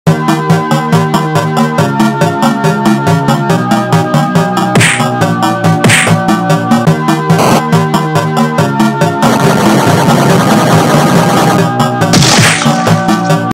scratch battle:slender man vs a-345 (a-345 in desc)
a-345 is intrminble rooms fanmade.
they is fast and high brain.